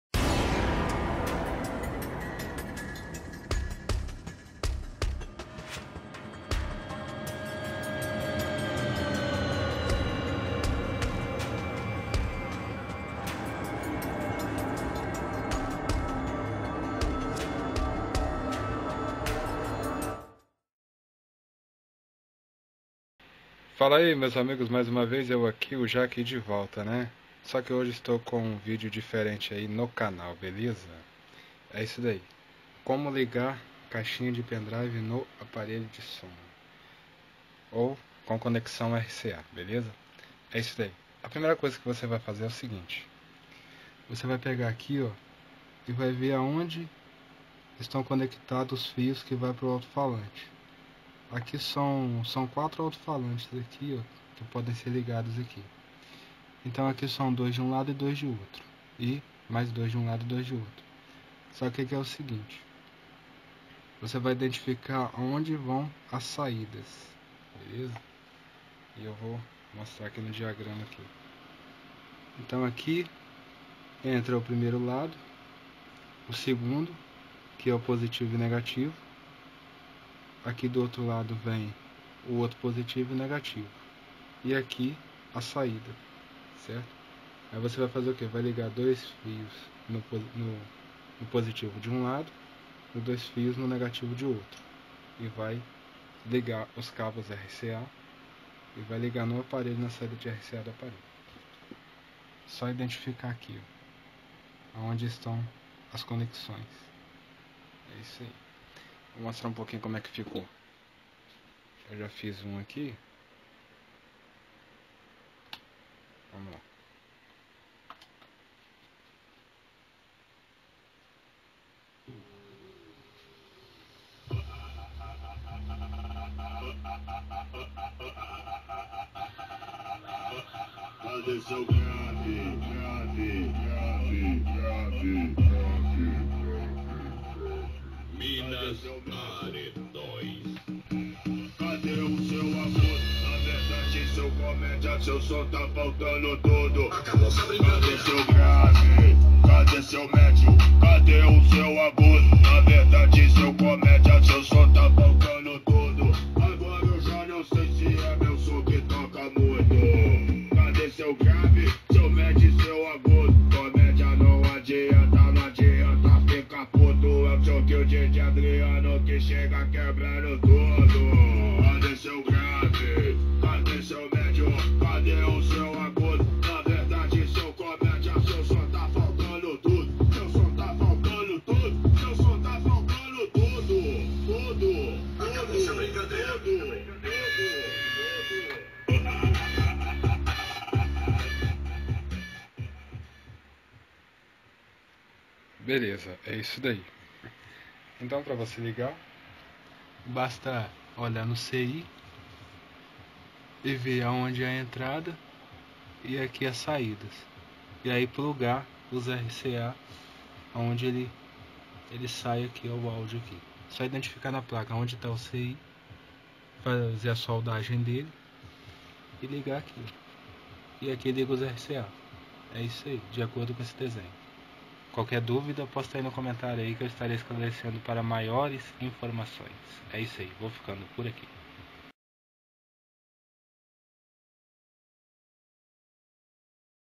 Como Instalar Caixinha de Pendriver No som (06 12 2019)
Fala e meus amigos video novo no canal, tudo blz ? venho com mas um video , como instalar caixinha de pendriver no seu som para ouvir os MP3 , com pouco de trabalho mas ,valido pois fica muito bom , vamos acompanhar o video e ver como ficou a ideia.